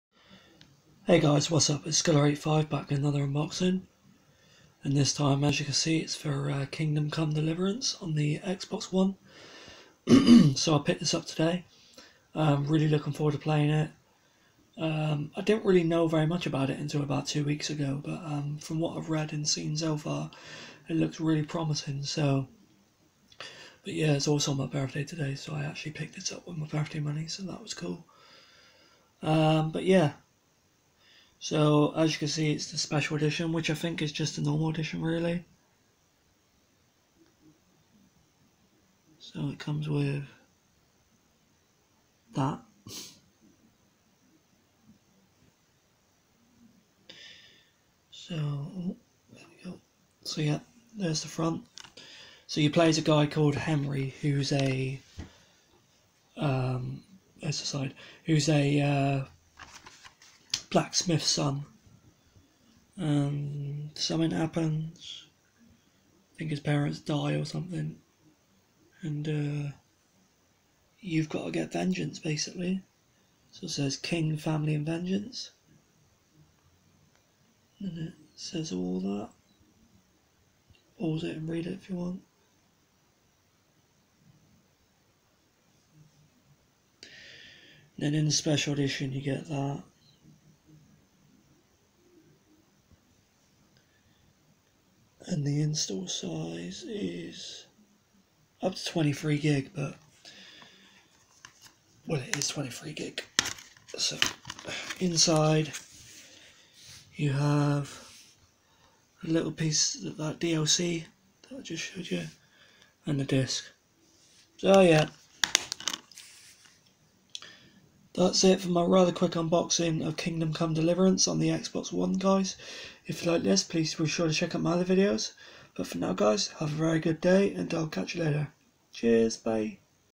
Kingdom Come: Deliverance (Xbox One) Unboxing
Hi there, today i unbox Kingdom Come Deliverance for the Xbox One.